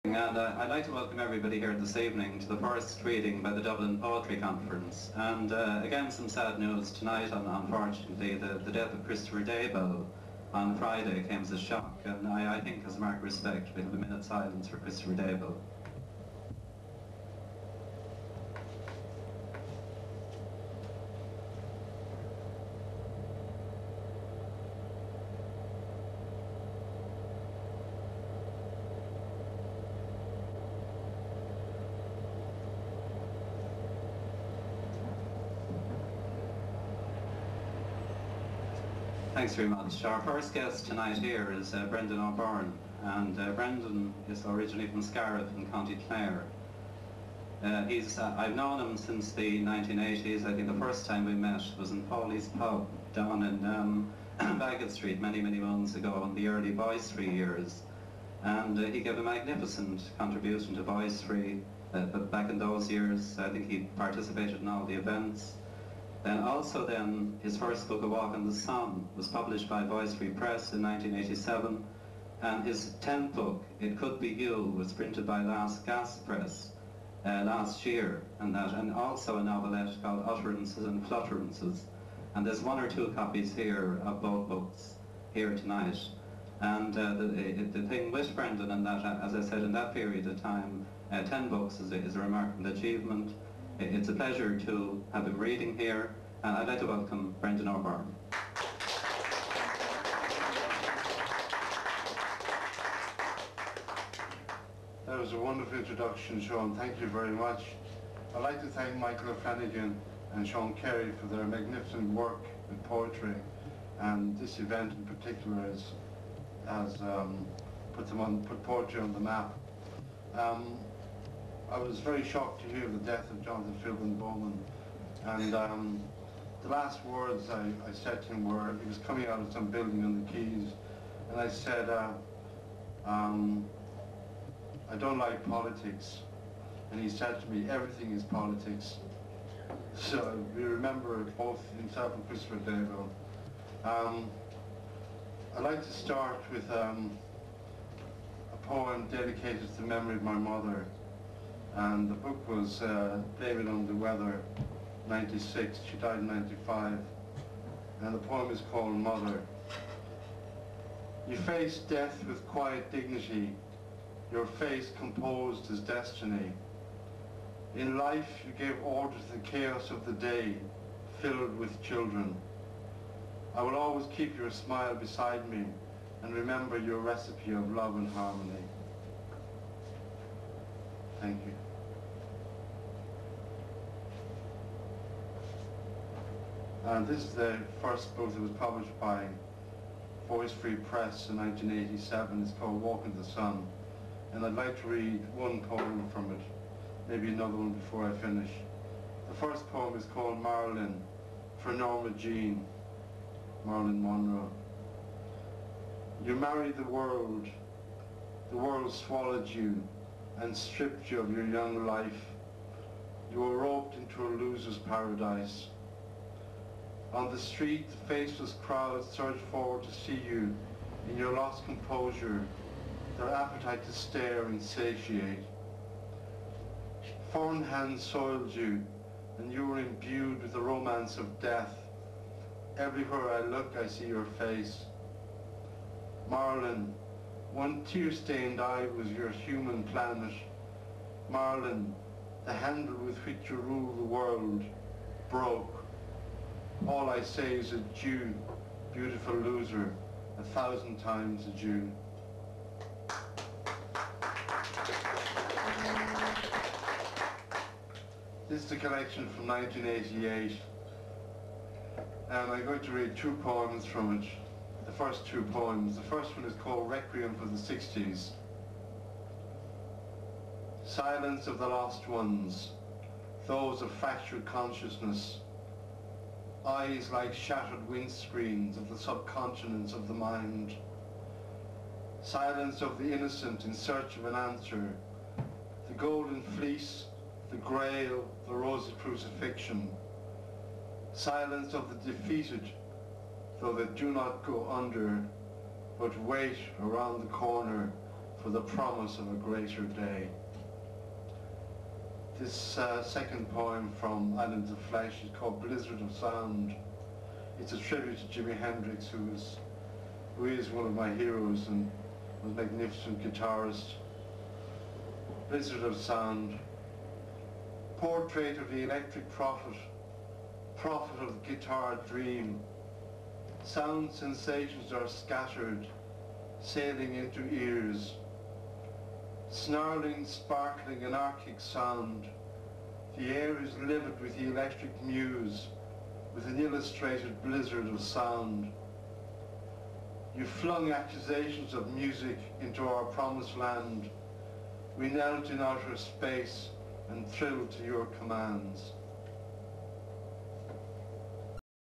Dublin Poetry Conference Brendan O'Berine
Brendan O'Berine reads his poems at the Dublin Poetry Conference in Bowes 1999.